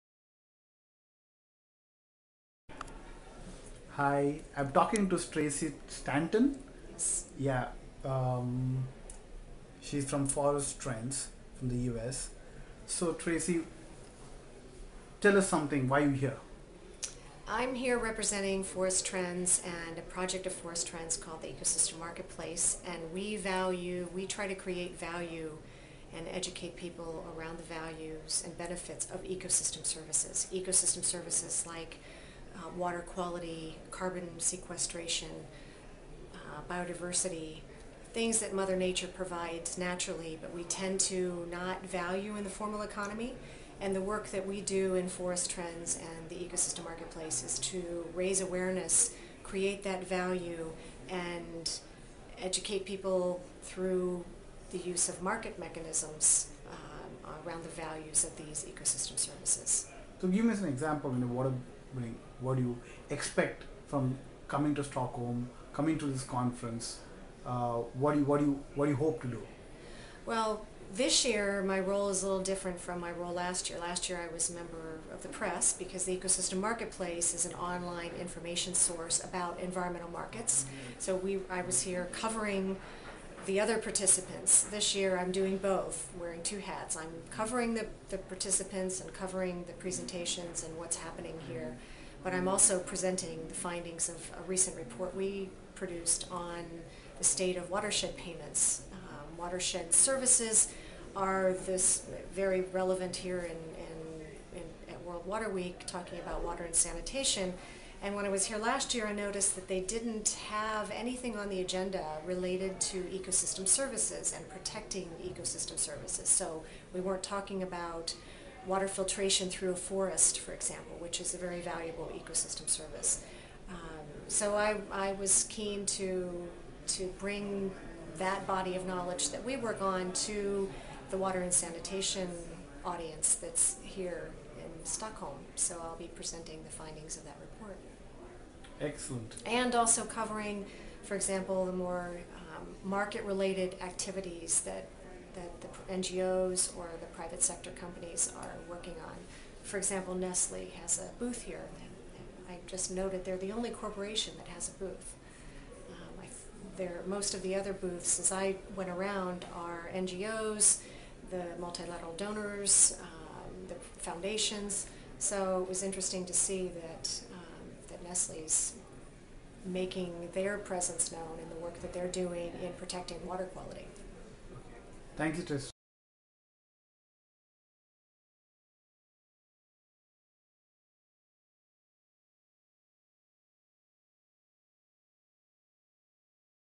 Tracy Stanton -Forest Trends / Ecosystem Marketplace
A conversation between Tracy L. Stanton, Water Program Manager, Forest Trends and Ecosystem Marketplace and Deepak Menon, Sr. Manager, India Water Portal. (Sep  6, 2010)